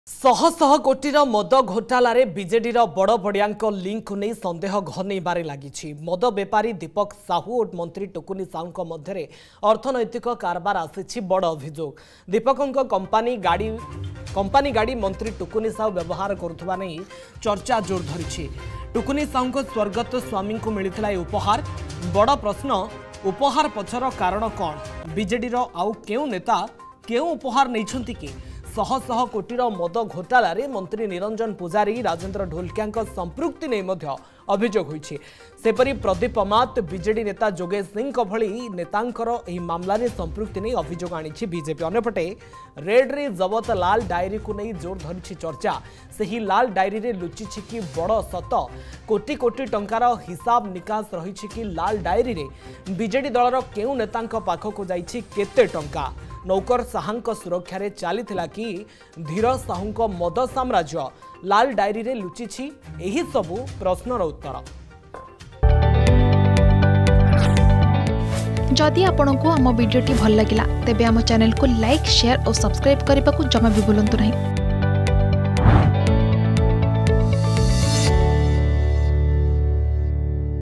The BJD Is Suspected Of having Link In The Liquor Scam Worth Hundreds Of Crores | IT Raid | Balangir
The BJD Is Suspected Of having Link In The Liquor Scam Worth Hundreds Of Crores.

ଶହ ଶହ କୋଟିର ମଦ ଘୋଟାଲାରେ ବିଜେଡି ଲିଙ୍କ ! || IT Raid || Balangir || Odia News